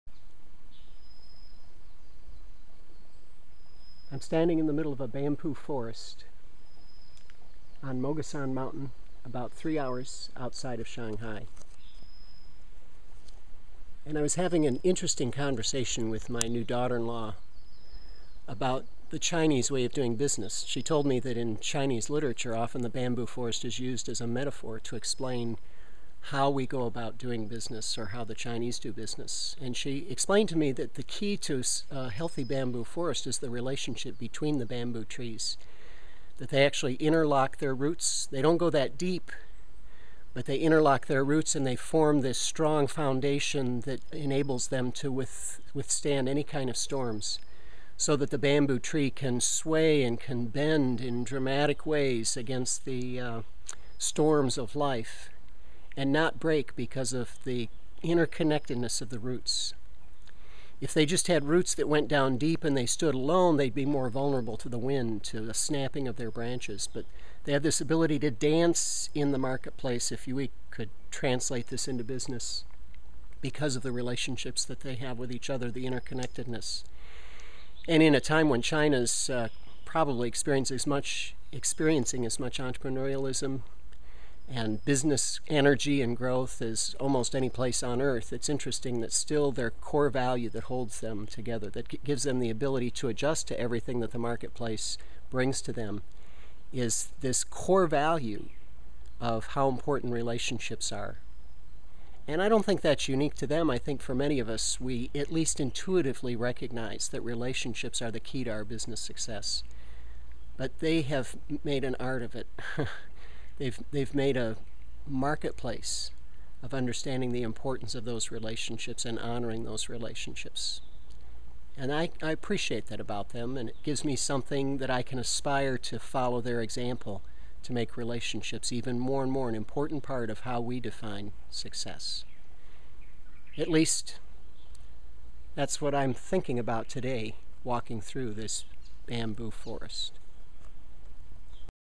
Lessons from a Bamboo Forest
Watch as Ron Price takes you to a bamboo forest outside of Shanghai, China.  He shares the similarities of this forest and a healthy business and business relationships.